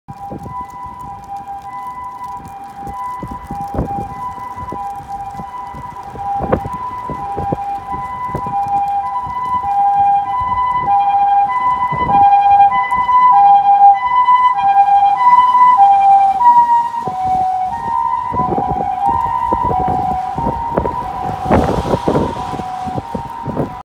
札幌市消防局 中央救急 緊急走行 Sapporo Fire Bureau Ambulance
An ambulance responding along South 4.